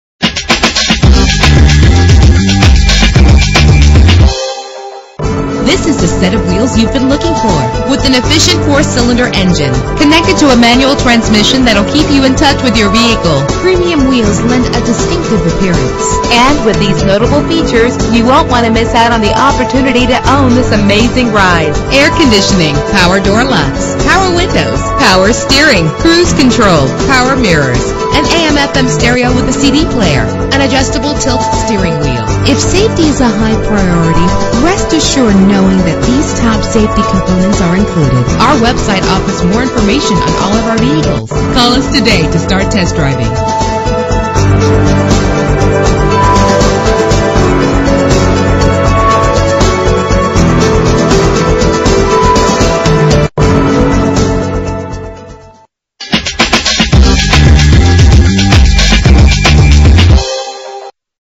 1993 Toyota Camry - Everett WA
Year: 1993
Make: Toyota
Model: Camry
Trim: LE
Engine: 2.2 liter inline 4 cylinder 16 valve
Transmission: 5-Speed Manual
Color: Gold
Mileage: 116358
Address:
9815 Evergreen Way